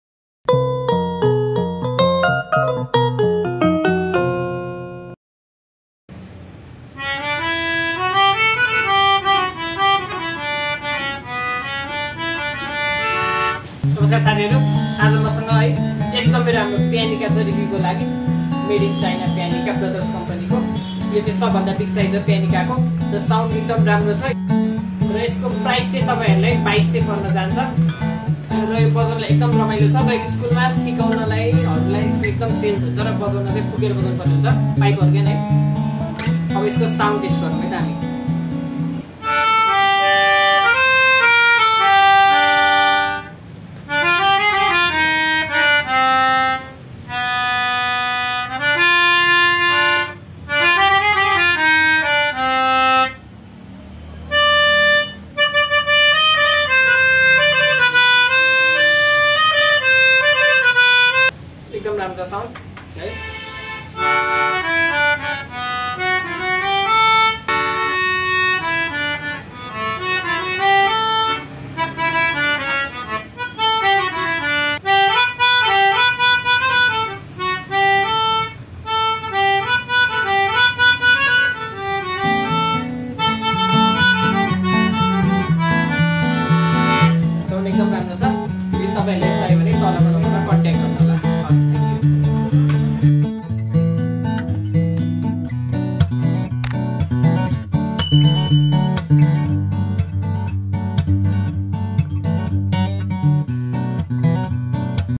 Pianica price in Nepal || Melodica review || Melodica sound
Contact or viber at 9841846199 Only Rs. - 2,200\-
Brothers company melodica.

A miniature keyboard powered by your own breath
Sound similar to a harmonica or accordion
37 key 3 octave range (F3-F6)

Includes both a fixed mouthpiece and a flexible air tube.